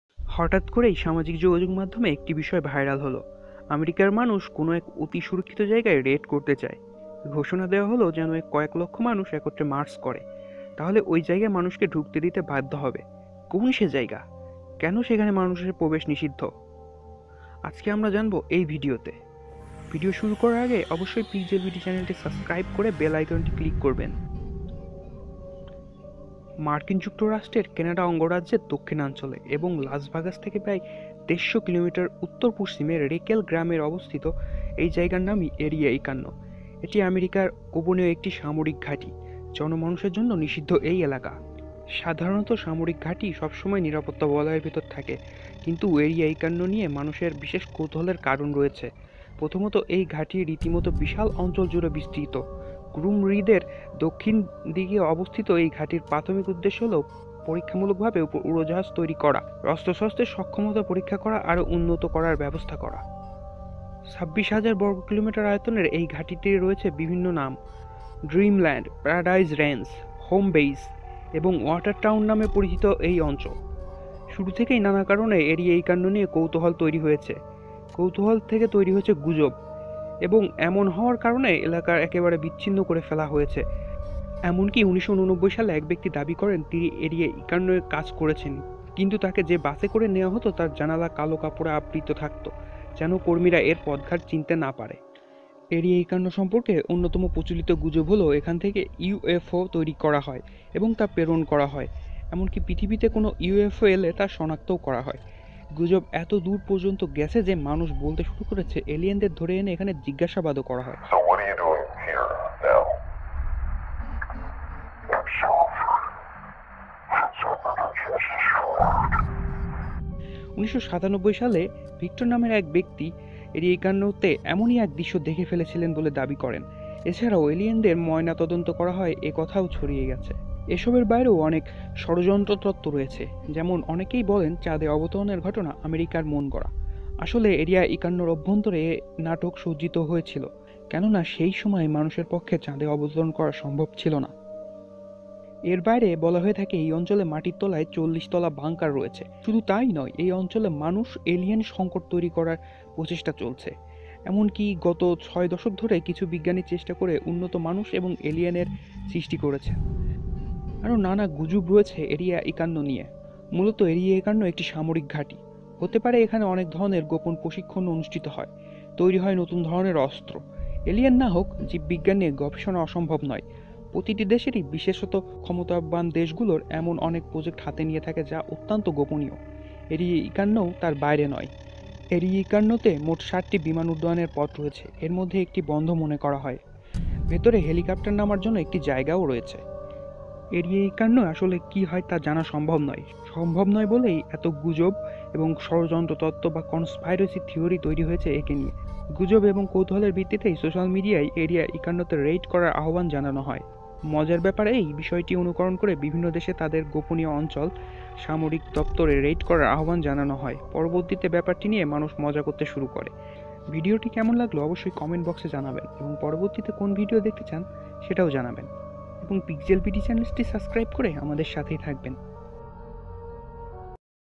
এরিয়া ৫১ এর রহস্য | এলিয়েন দের এখানে ধরে রেখে কি করা হচ্ছে | Area 51 bangla
এরিয়া ৫১ সম্পর্কে রহস্যময় ও রোমাঞ্চকর তথ্য উঠে এলো গবেষণা